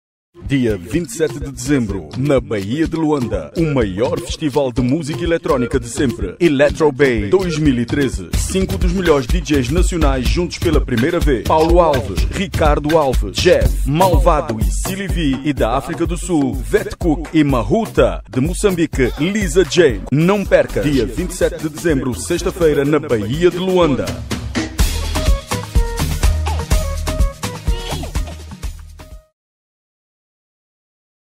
Electro Bay 2013
Dia 27 de Dezembro o Electro Bay 2013, na Baía de Luanda, vai reunir 5 dos melhores DJ's de Angola - DJ Paulo Alves, Ricardo Alves, Dj Djeff Afrozila Fun Club, Malvado e Dj Silyvi - e ainda o melhor do Afro-House da África do Sul com DJ Vetkuk vs Mahoota e de Moçambique a Queen Lizha James.